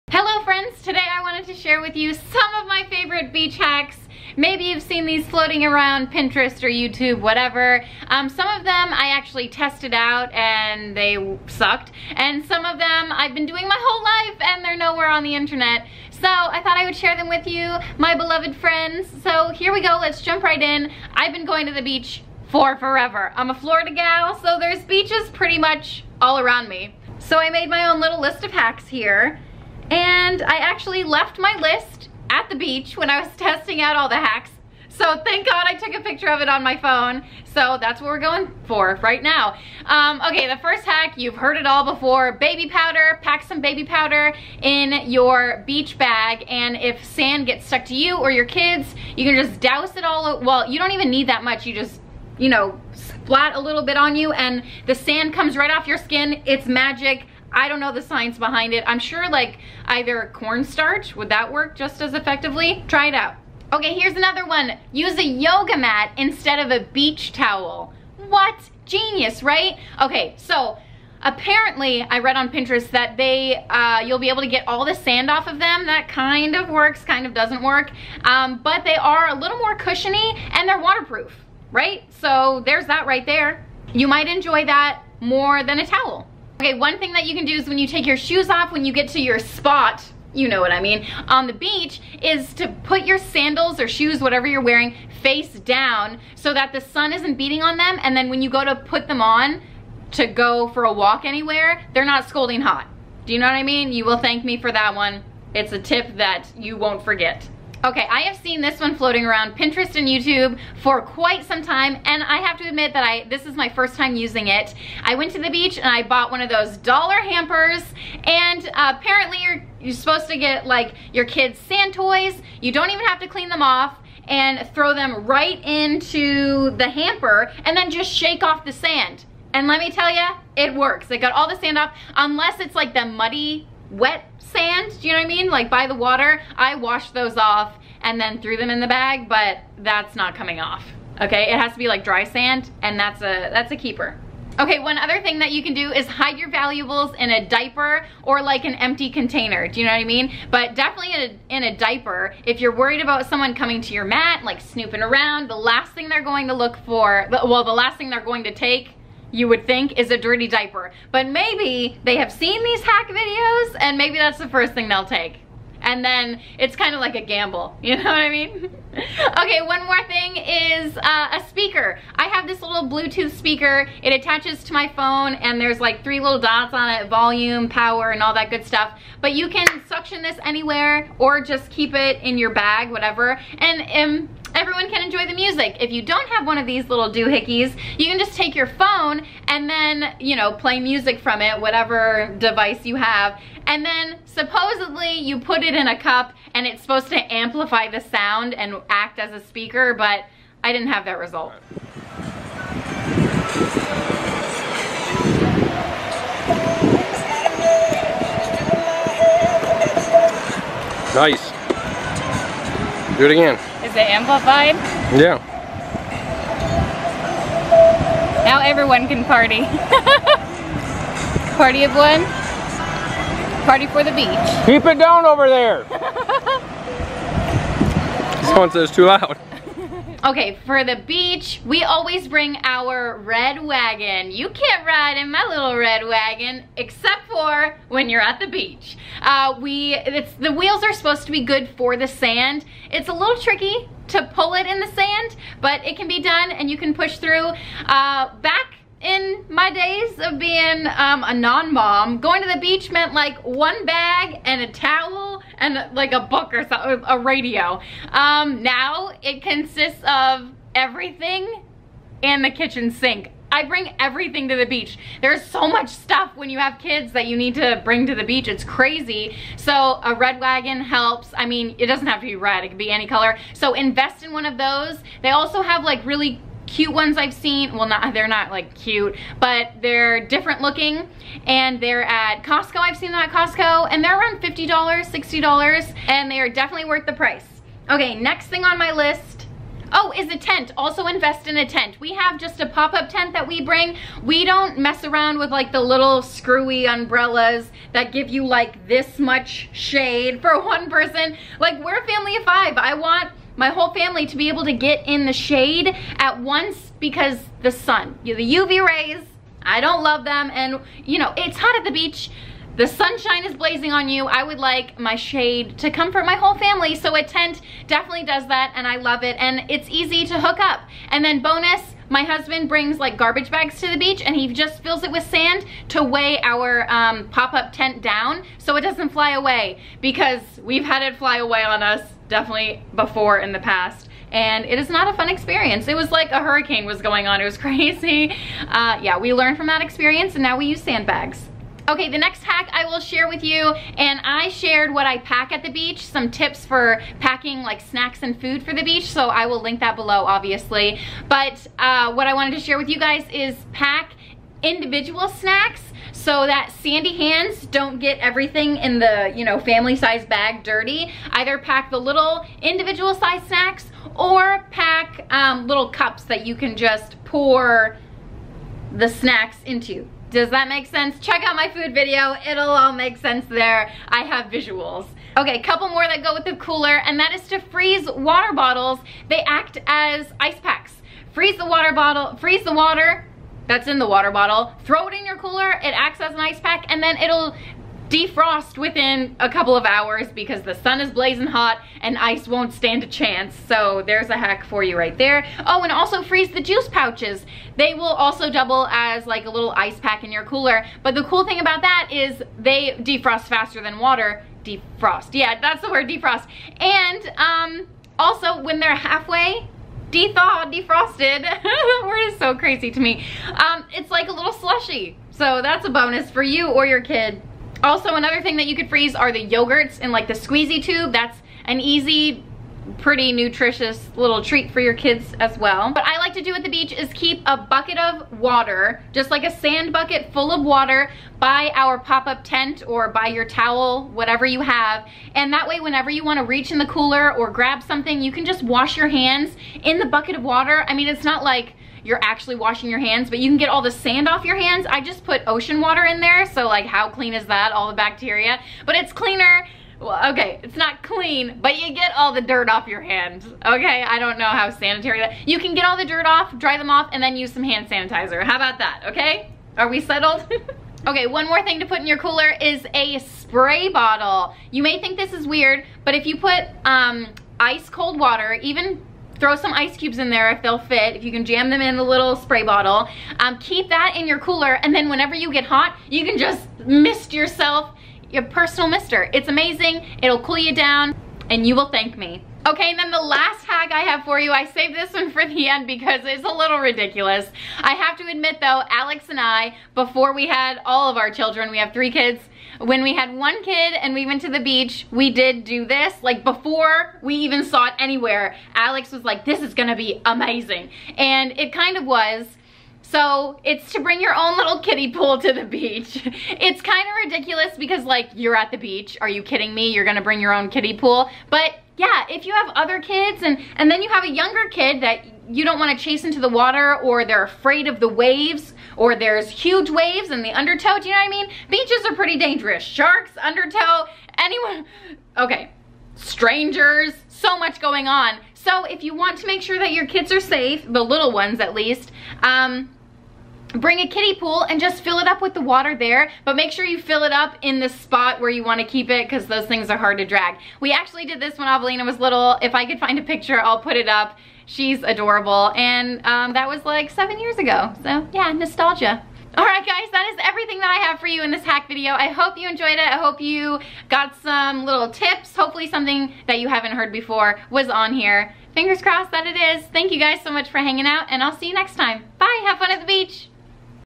Ultimate Beach Hacks! + Testing Some Out! Wish You Were At The Beach?? Me Too...
Hey Wad Squad!  I am sharing with you some of my favorite beach hacks!  These are some great ideas to put / pack with you for your next beach trip!  I love the beach and I hope these hacks are helpful for you!

Best,
  Kim!!